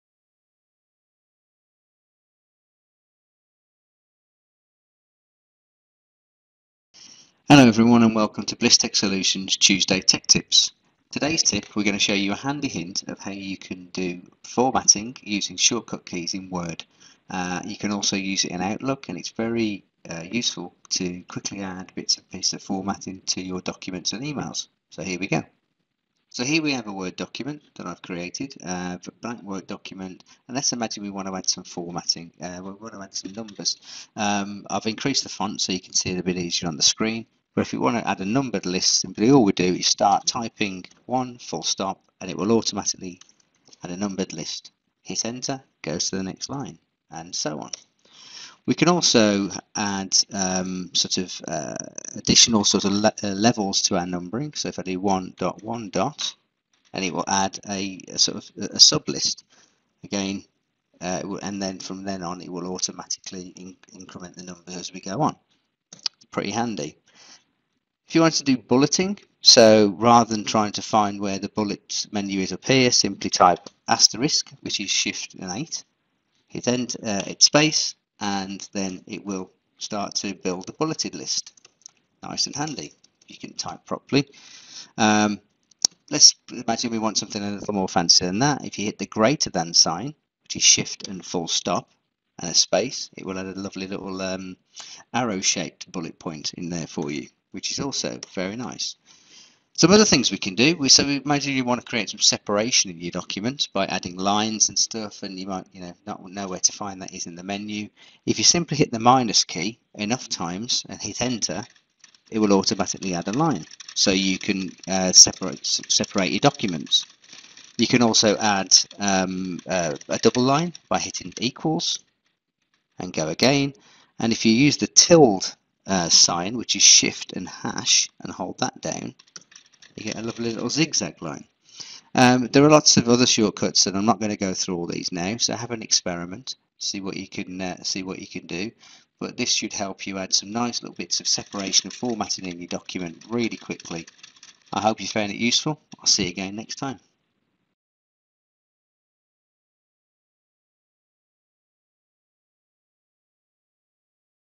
Quickly add lines, numbers and bullets in Word
Happy Tuesday everyone!  Today's Tech Tip is a tip on how to use some of the formatting shortcuts in Word to add lines, bullets and numbers to organise your documents.

If you use Word to write your documents, and 90% of people do, you will love this tip as it will show you how to quickly add formatting to speed up getting things done.

And as a bonus, these tips also work in Outlook! 👊💥

This week's tip will show you how to:

   👉 Quickly create a bulleted or numbered list.
   👉 Quickly add different styles of lines to your document.

Today's tip will help you to:

   💥 Stop hunting around the menu in Word to find things.
   💥 Speed up adding formatting and organisation to your documents.

We hope you enjoy it.  The link is in the first comment.